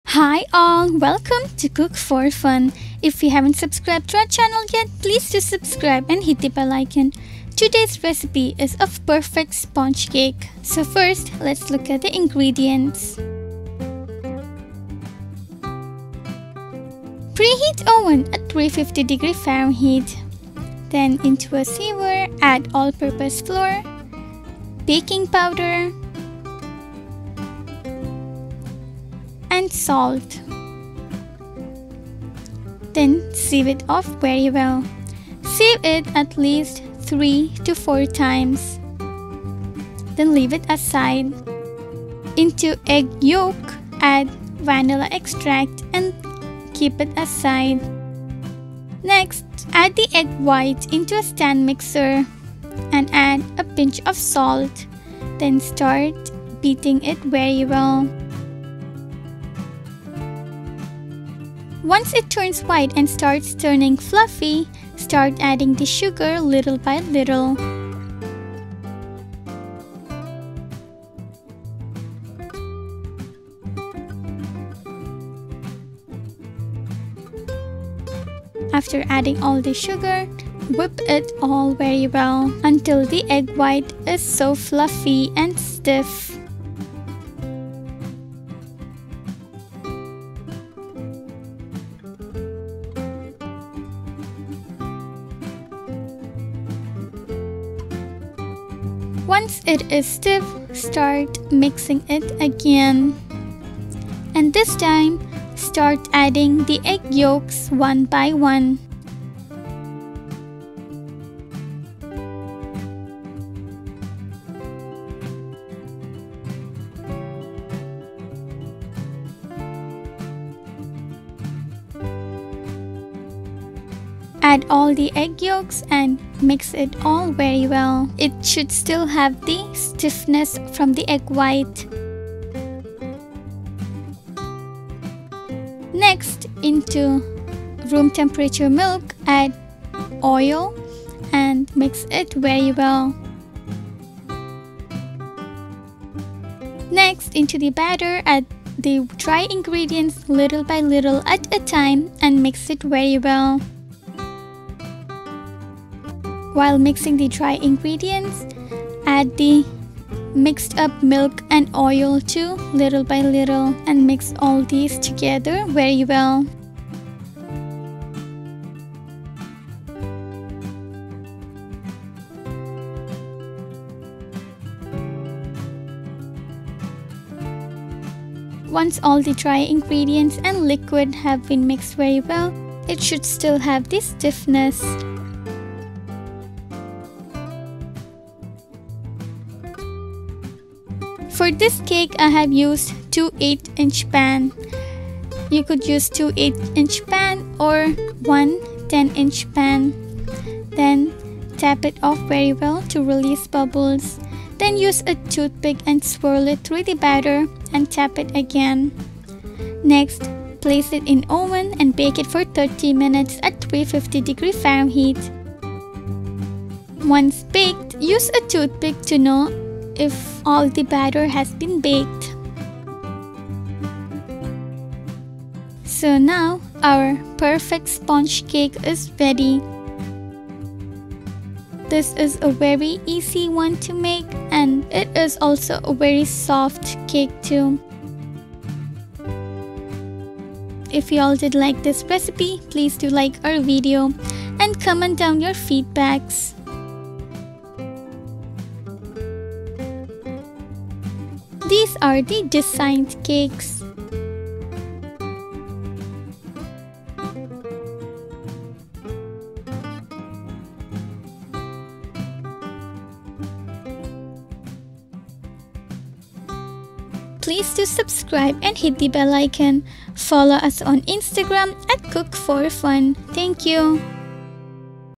Perfect Sponge Cake | Vanilla Sponge Cake
Sponge Cake

Room temperature egg yolk - 4
Vanilla extract - 1 1/2 tsp
Room temperature egg white - 4
Room temperature milk - 1/4 cup